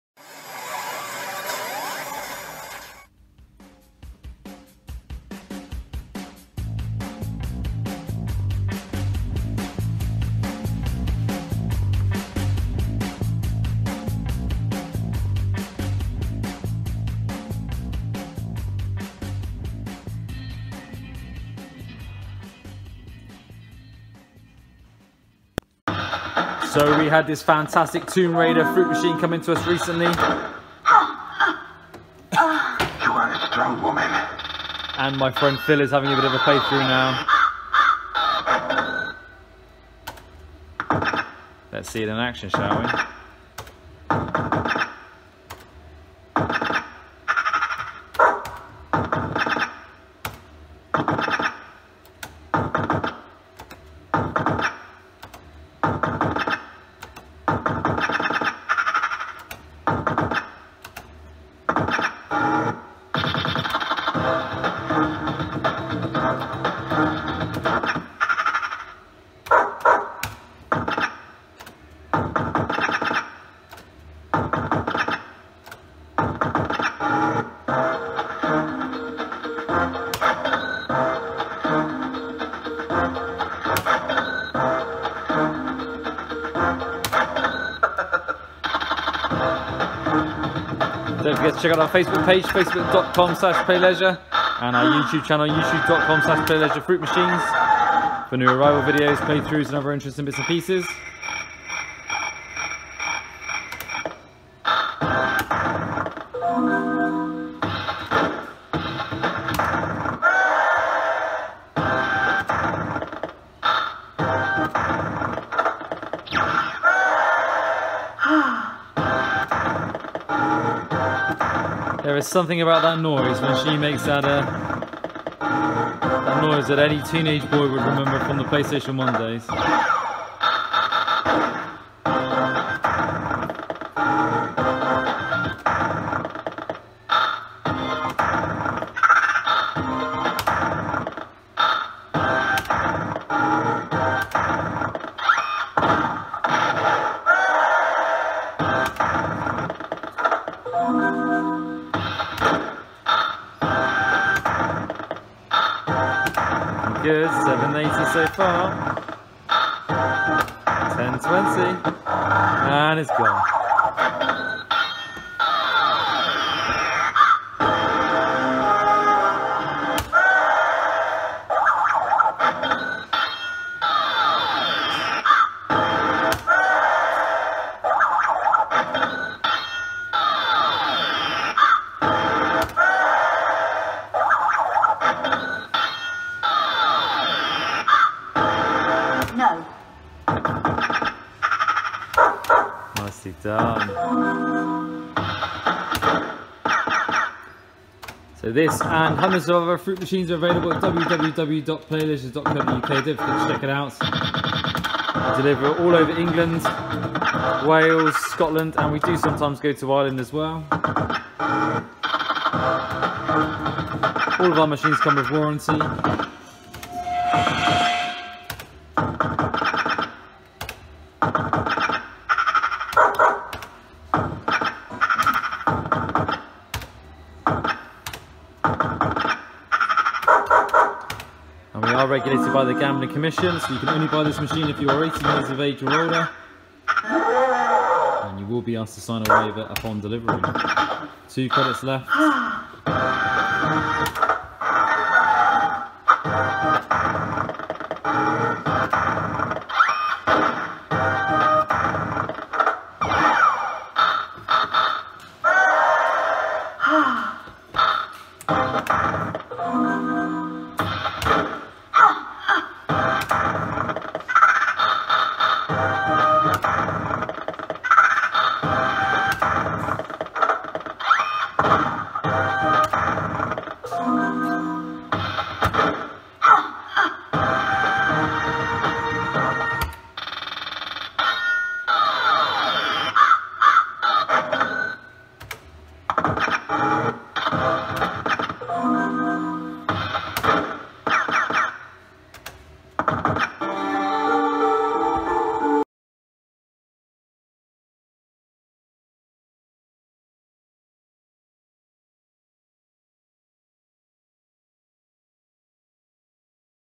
Phil Plays... The FANTASTIC Tomb Raider JPM Fruit Machine!
Our resident Fruit Machine Expert Phil spends a bit of time playing the fantastic Tomb Raider JPM Fruit Machine in real play mode, enjoy!

Remember, we stock hundreds of Fruit Machines and all of our machines are fully serviced and include warranty.